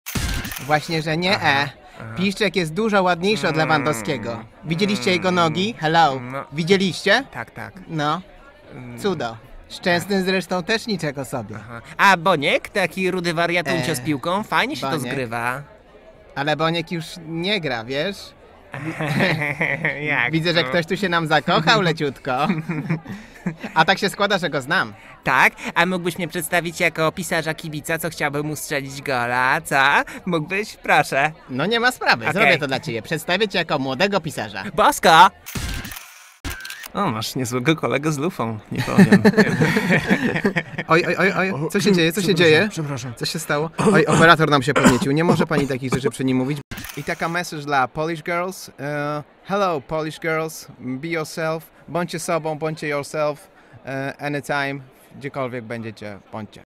Michał Witkowski - Chciałbym strzelić Bońkowi gola! - Ścianka Myśli
"A mógłbyś mnie przedstawić jako pisarza-kibica, który chciałby mu strzelić gola?"
Michał Witkowski uczestniczył w dyskusji dotyczącej urody piłkarzy. Okazuje się, że najbardziej podoba mu się Zbigniew Boniek, któremu chciałby strzelić gola.